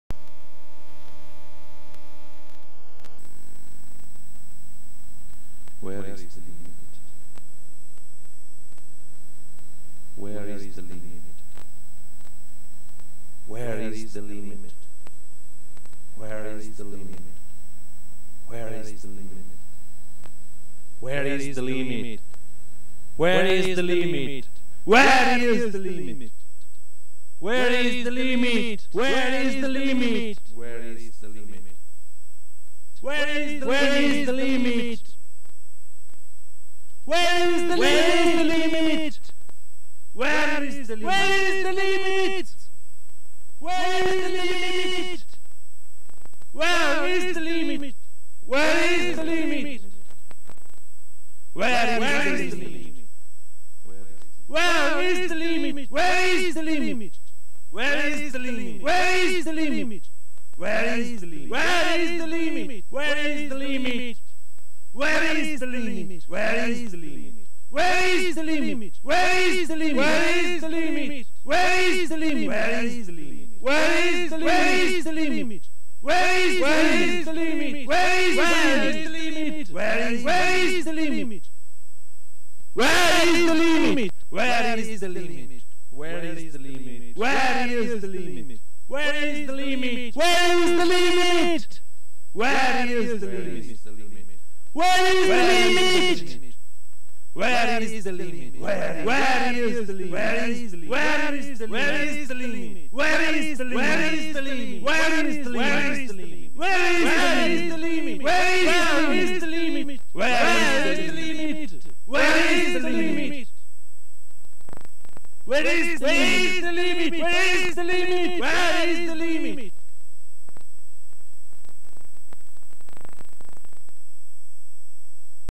Where Is The Limit?
Videoart for multimedia installation.
No answer for the question: WHERE IS THE LIMIT?
Federico Bucalossi | Nothuman
1999 Hi8 + Digitaal Flash